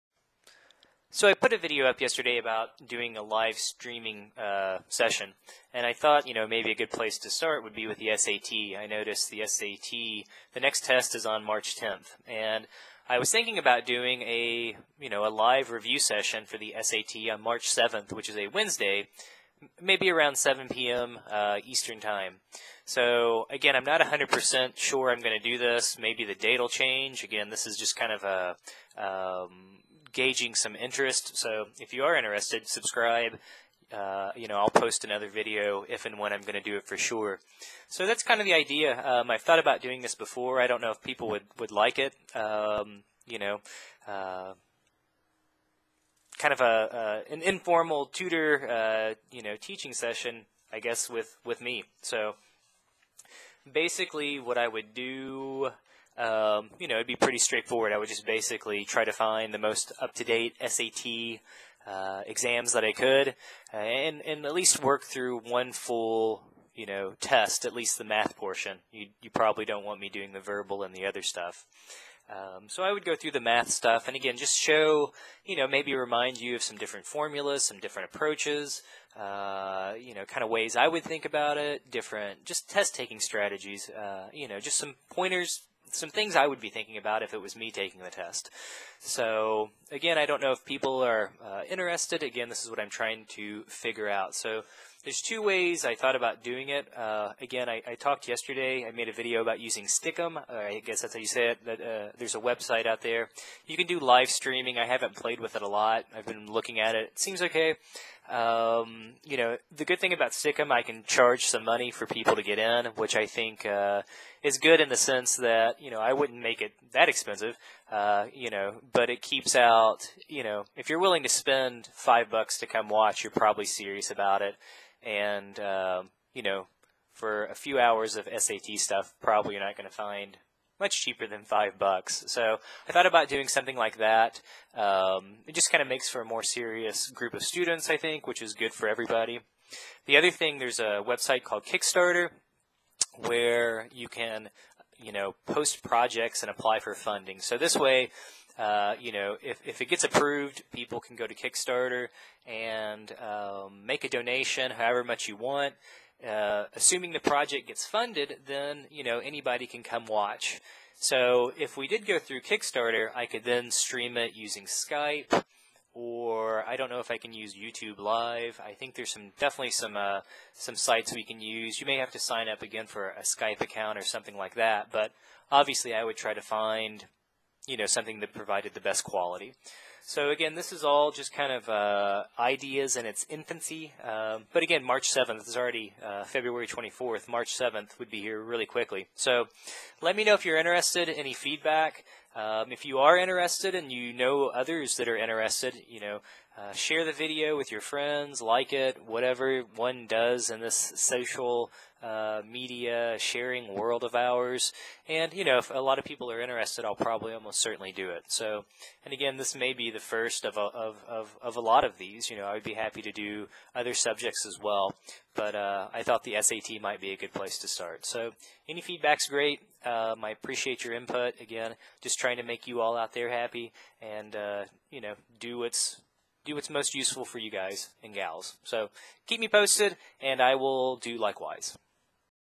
Live Streaming SAT Test Review on March 7th at 7pm Eastern Time?!
You da real mvps!   $1 per month helps!! Live Streaming SAT Test Review on March 7th at 7pm?!  Would you all be interested in a live streaming SAT review by me on March 7th?  I would work through a practice exam and show how I would approach the problems.  I assume it would at least last a couple of hours.
Any feedback and input would be great!